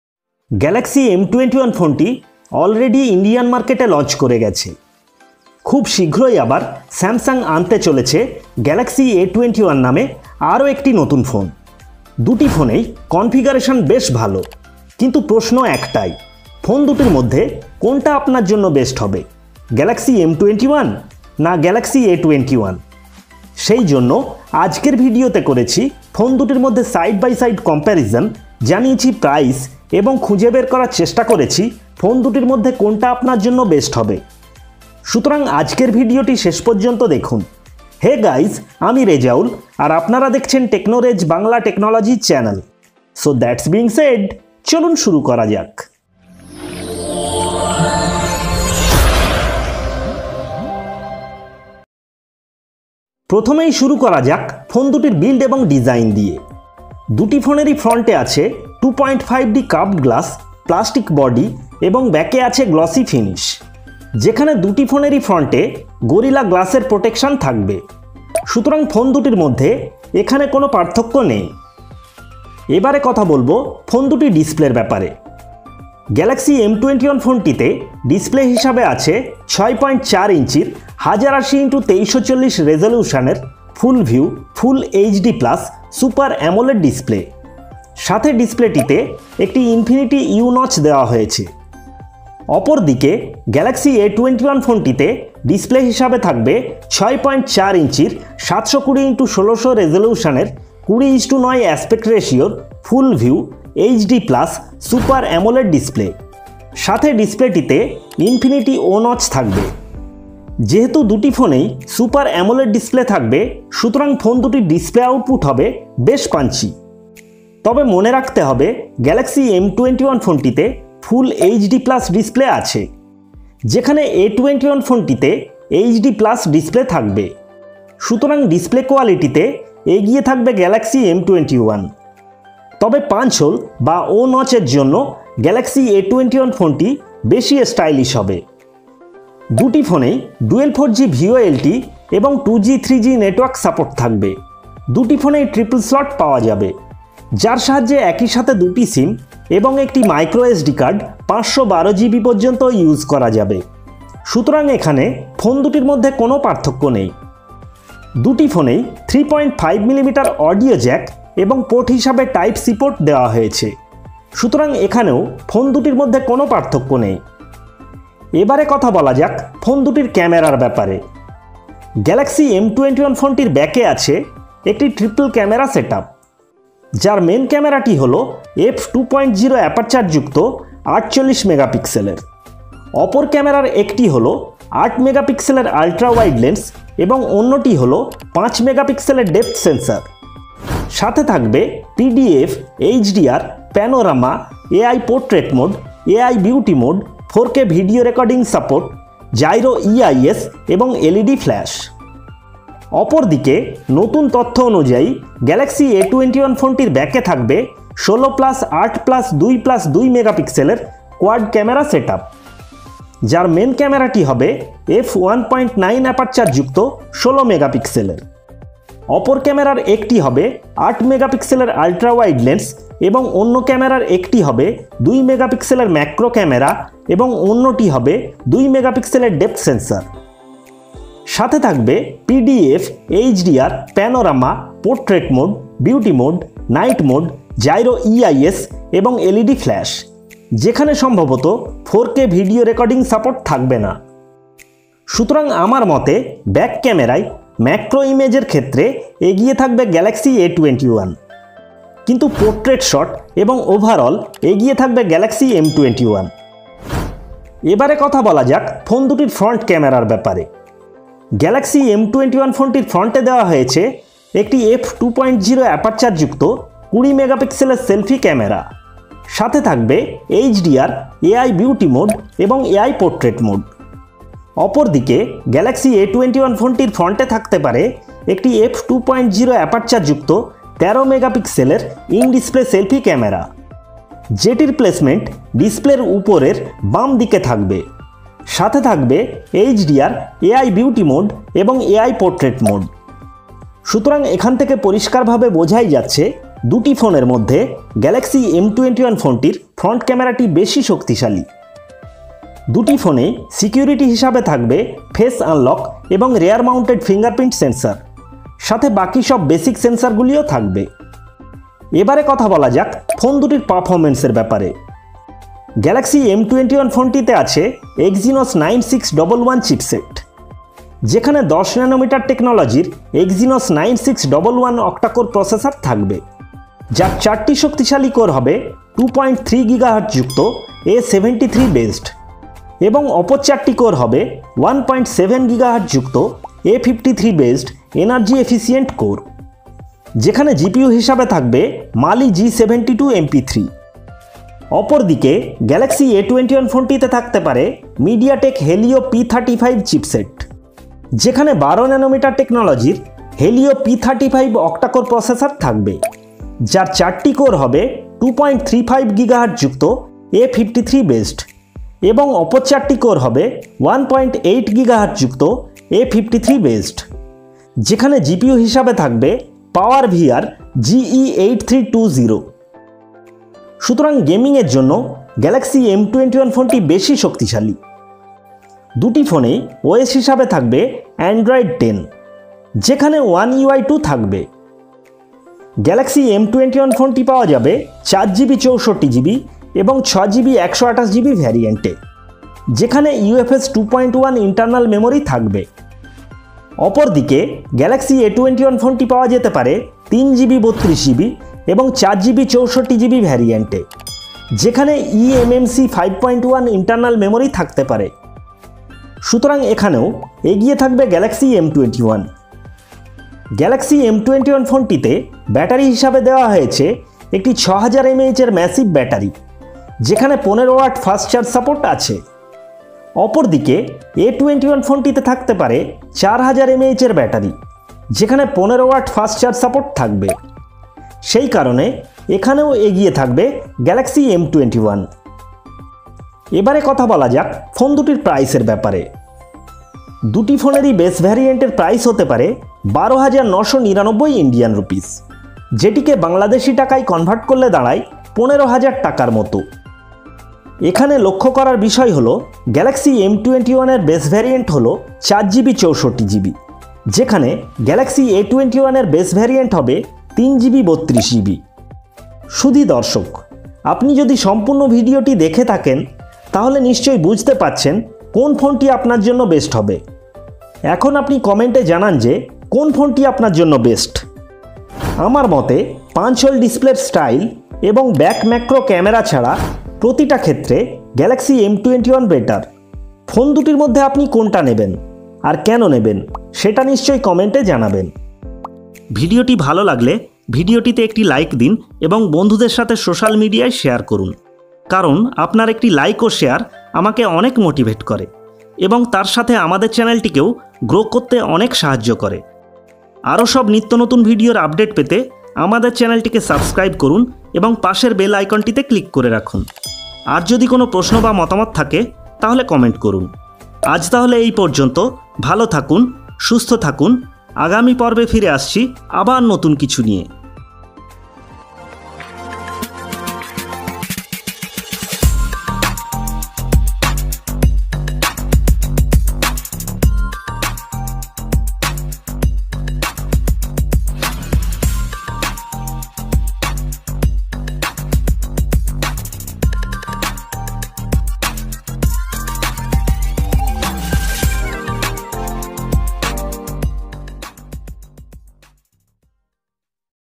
Compare Samsung Galaxy A21 vs Galaxy M21 Bangla | Specs, camera, Price|My Honest Opinion & Review
Hello Guys welcome to "Techno Rez" Today I am Going to make a Side by side comparison and review of Upcoming Samsung Galaxy A21 Vs Samsung Galaxy M21 in Bangla. In this Video you will get full specification of these two phones. There are alot of confusions about Samsung Galaxy M21 Vs Galaxy A21 which will comming very soon, so in this video I am trying to clear that confusions. at the end I will give you my honest Opinion and you will also know the price in India and price in bd of Galaxy A21 & Galaxy M21, which will surely help you to decide which one to buy . So Please Watch the Video till the end otherwise you will not understand. I think you will Like the Video very much.

*The model concept is created by us with the basis of Official information & imagination. the actual model may or may not be the same. We can not guaranty the specifications are 100% acurate, it is based on trusted information from trusted sources.*
✌This is not a paid or biased review. Its my             Honest opinion.

Used video Credit: Official Mideatek Helio P35 Video, Oficial Samsung Exynos Video, Official Samsung Video & cc videos.